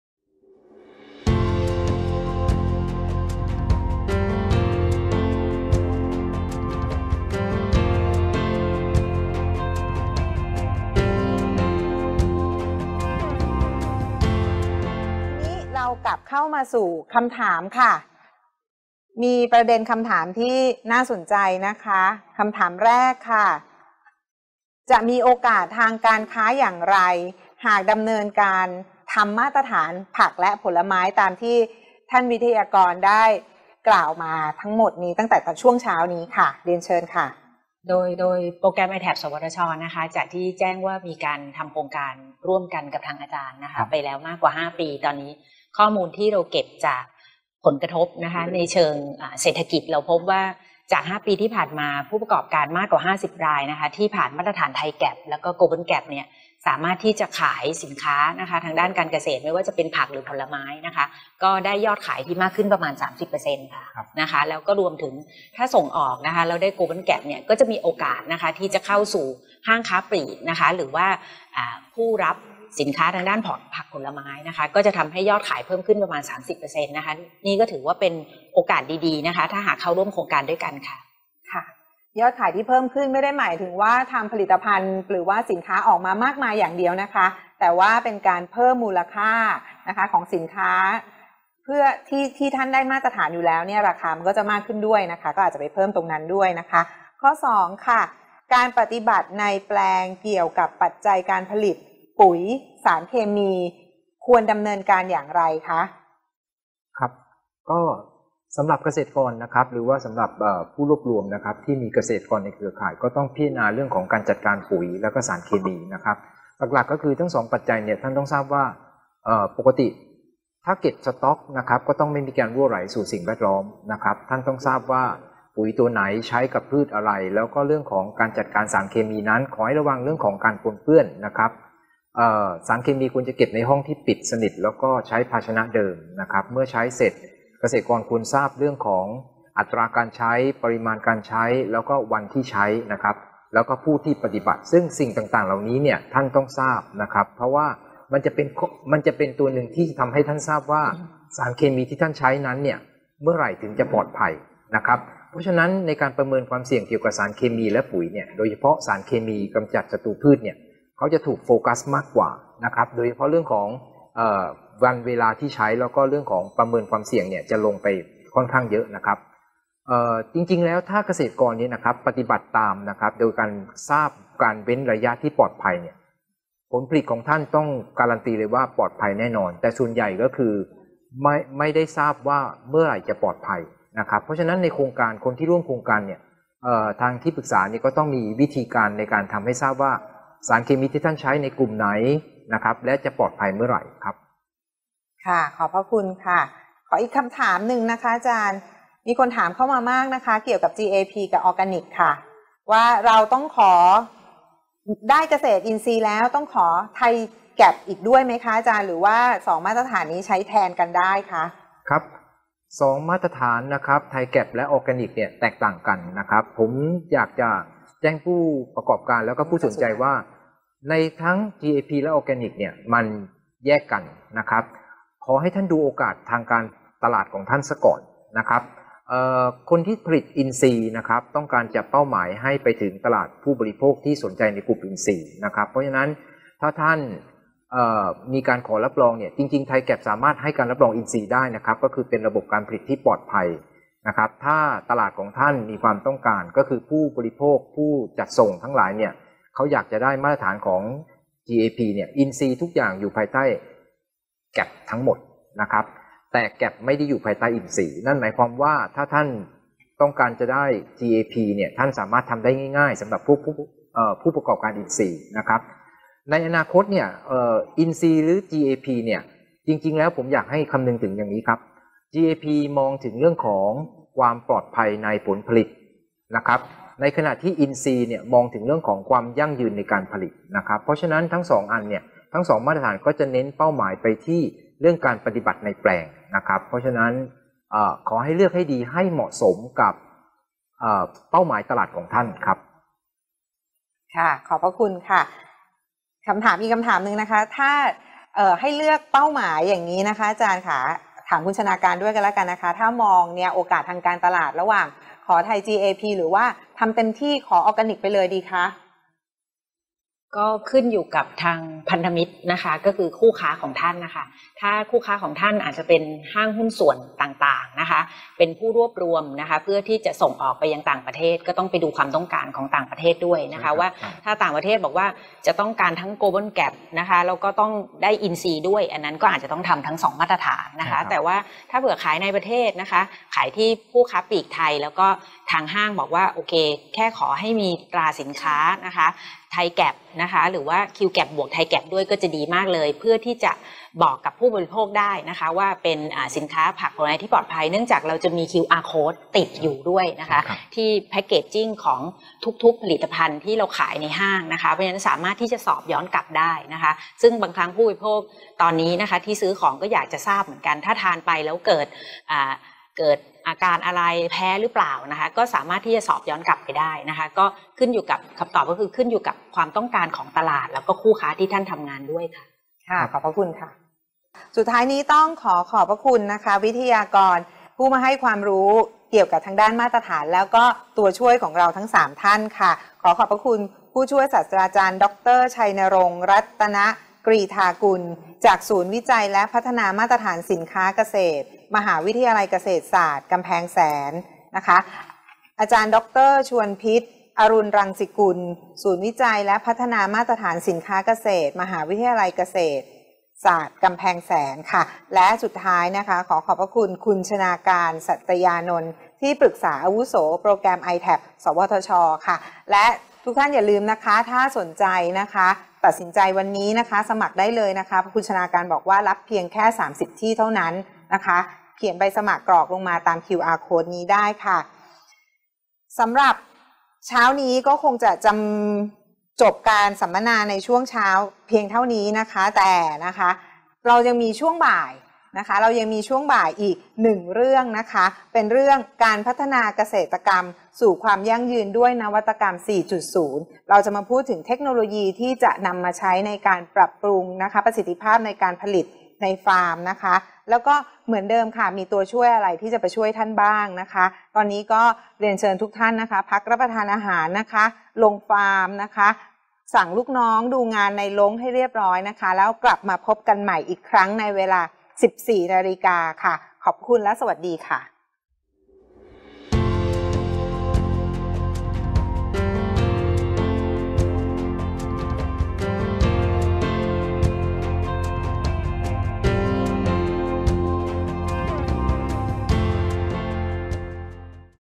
การสัมมนาออนไลน์หัวข้อ “การยกระดับผักและผลไม้ไทยสู่มาตรฐานสากล” (5/5)
ถาม-ตอบ และเปิดรับสมัครผู้สนใจเข้าโครงการสนับสนุนเงินทุนผู้ประกอบการ SME

การสัมมนาออนไลน์หัวข้อ “การยกระดับผักและผลไม้ไทยสู่มาตรฐานสากล” วันพฤหัสบดีที่ 4 มิถุนายน 2563 เวลา 10.00 – 12.15 น. จากอุทยานวิทยาศาสตร์ประเทศไทย สวทช. จ.ปทุมธานี